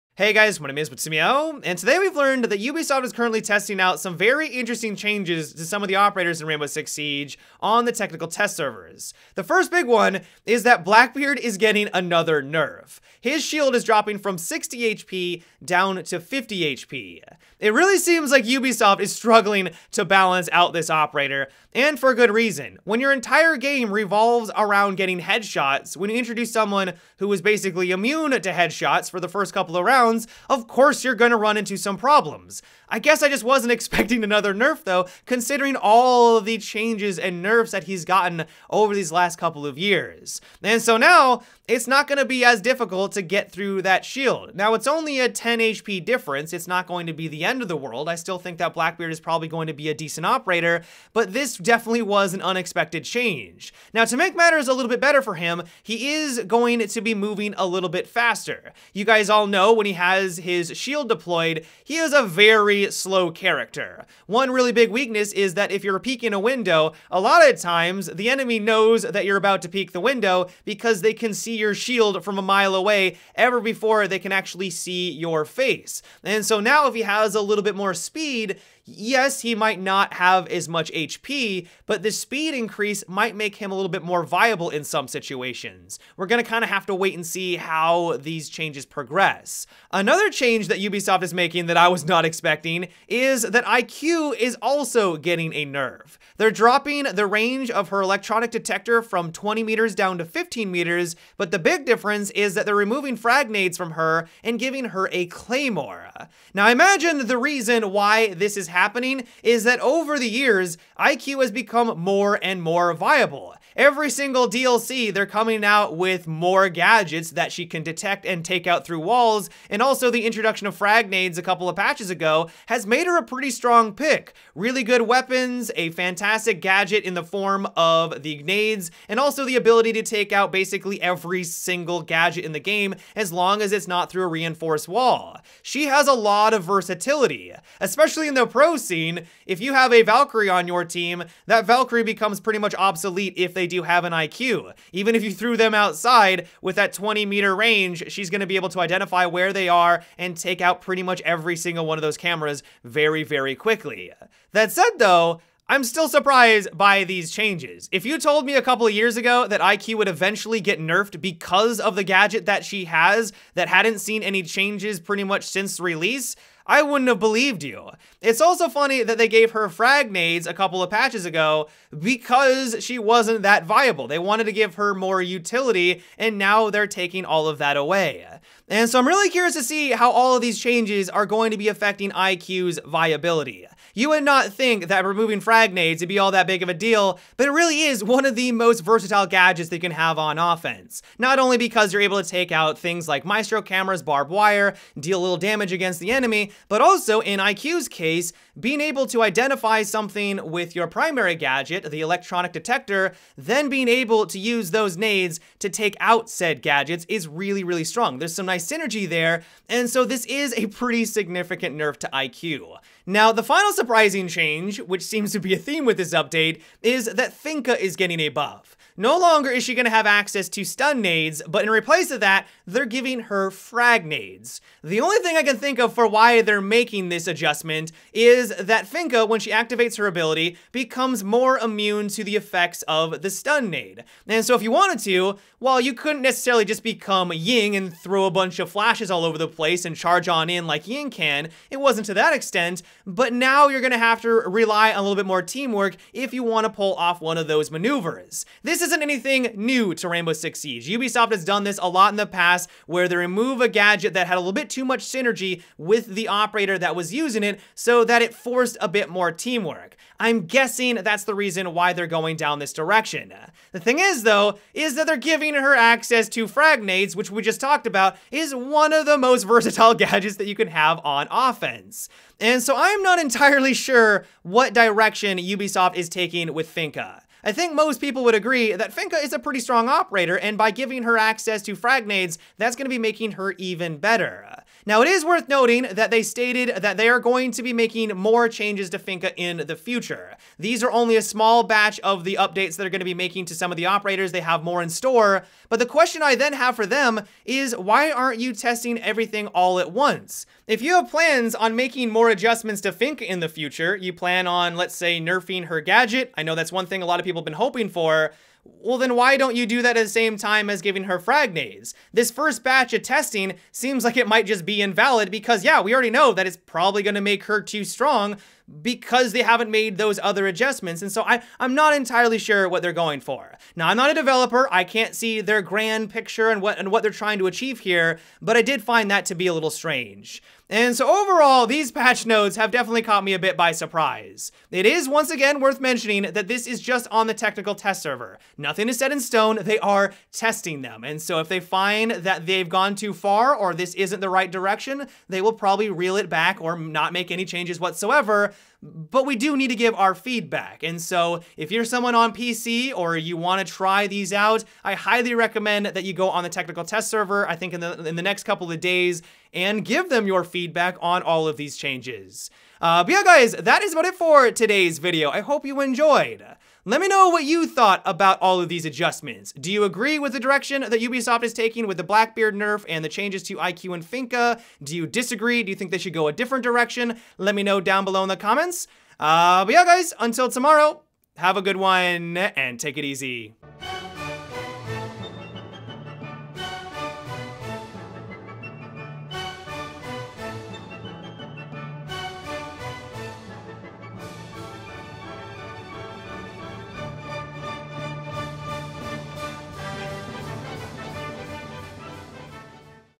Blackbeard Nerfed Again - Rainbow Six Siege Update
Today we learn that a few operators are getting changes soon in Rainbow Six Siege.